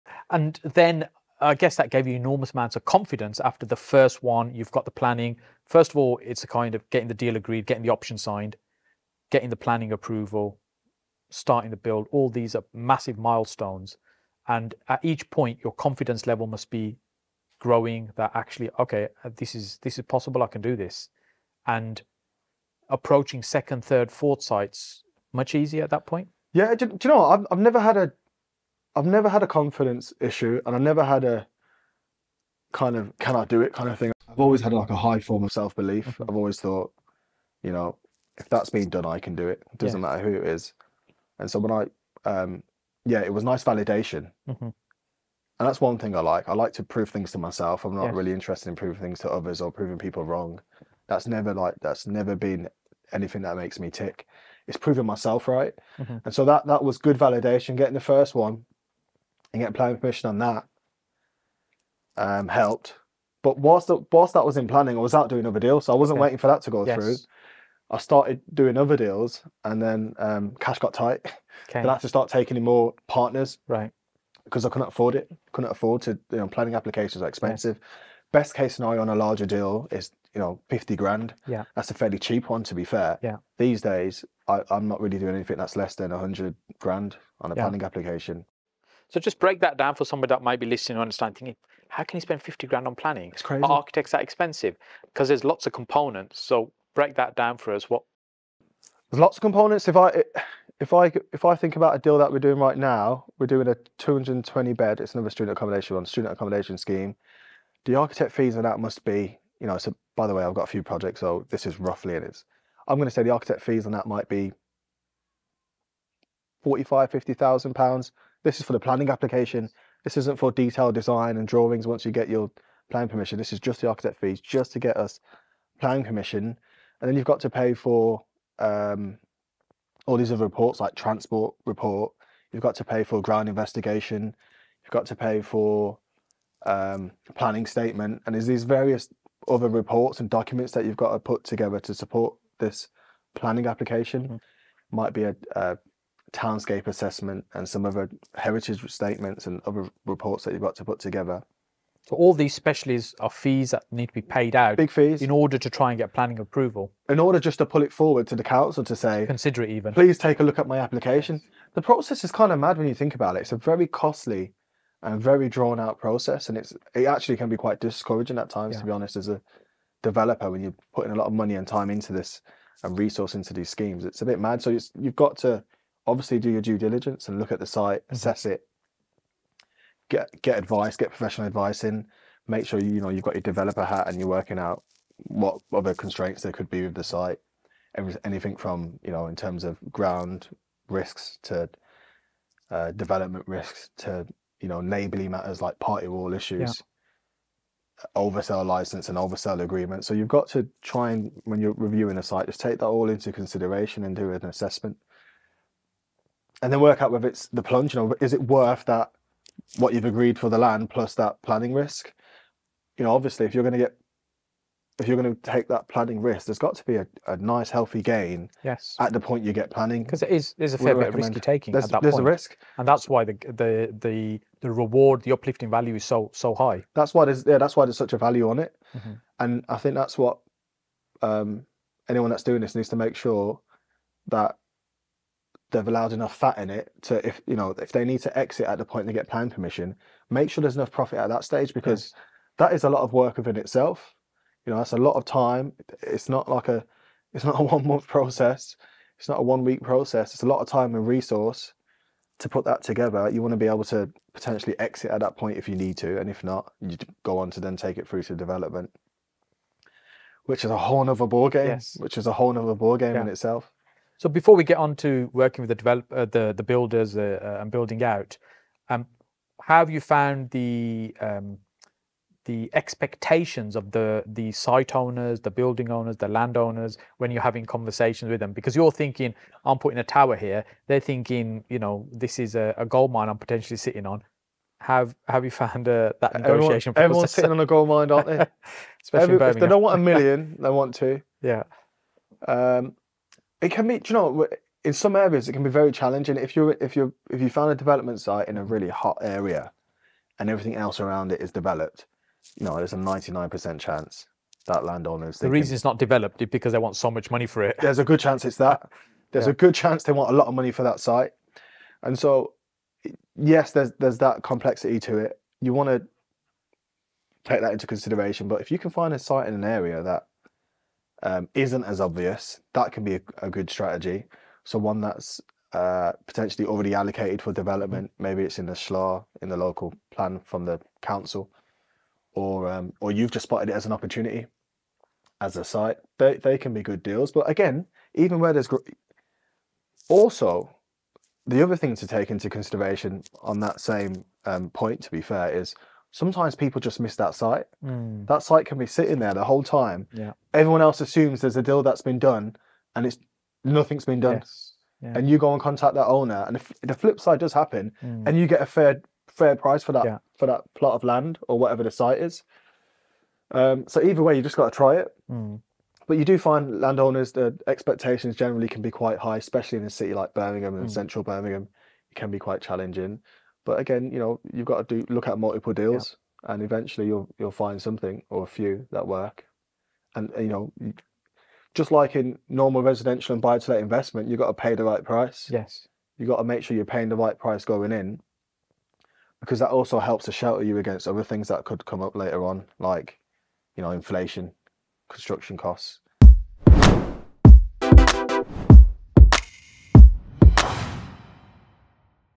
Spending £100,000 on Planning Application For Property Development w/Anthony Laville
Anthony Laville shares his story of being a Property Developer, Co-Living, and providing Student Accommodation before moving on to Deal Sourcing, Property Development, and continuing to grow his portfolio.